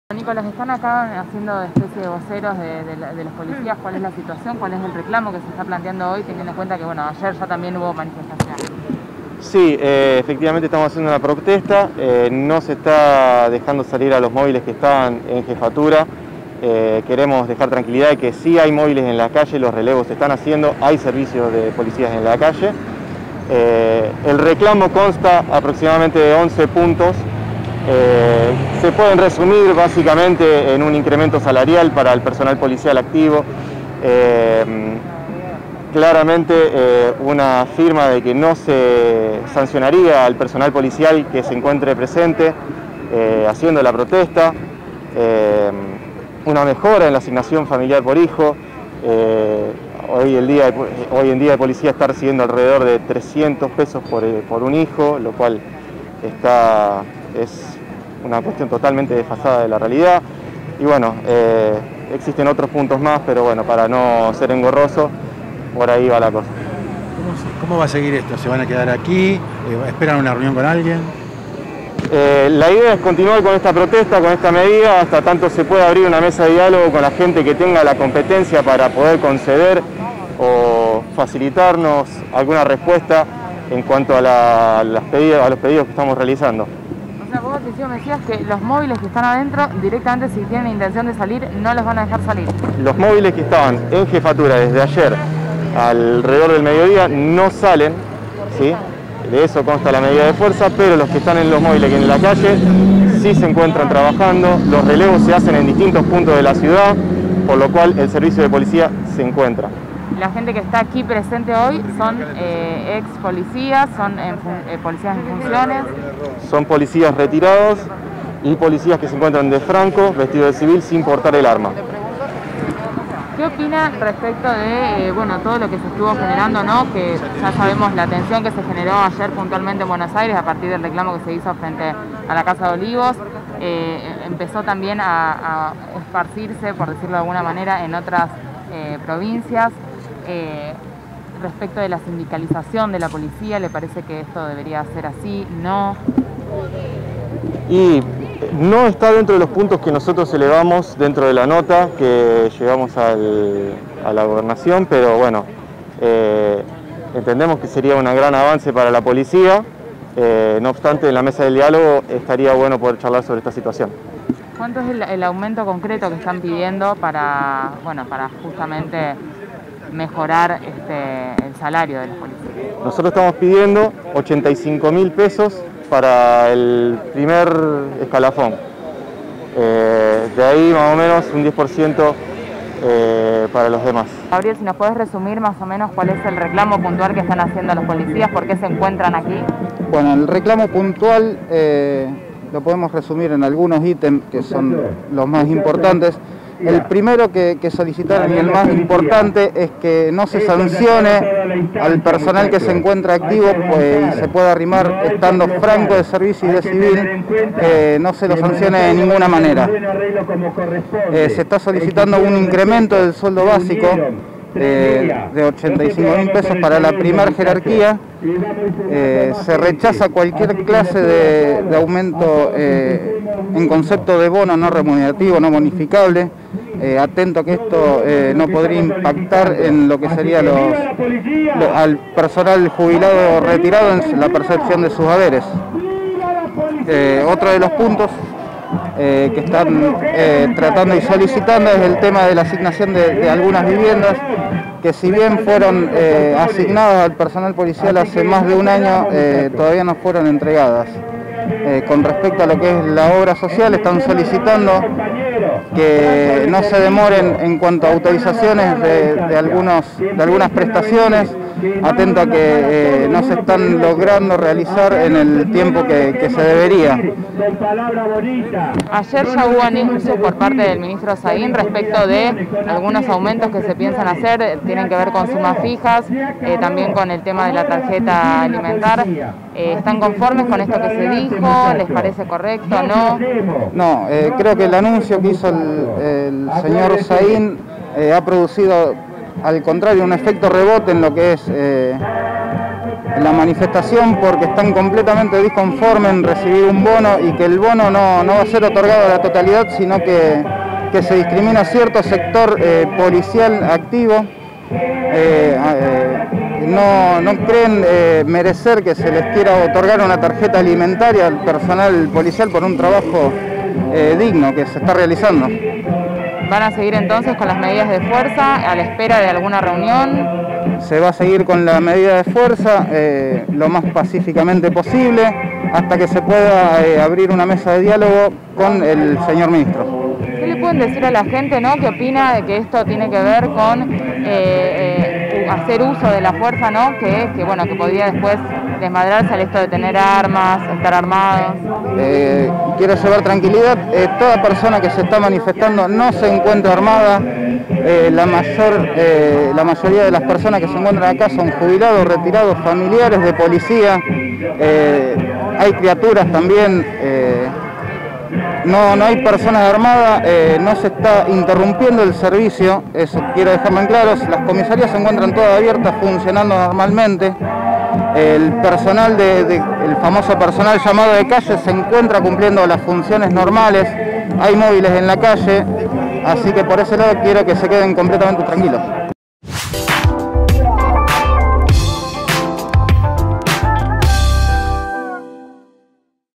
Familiares de policías y agentes retirados continúan reclamando mejora salarial
Los manifestantes no permiten la salida de móviles de la Jefatura de la Unidad Regional II y exigen que se conforme una mesa de dialogo para destrabar el conflicto.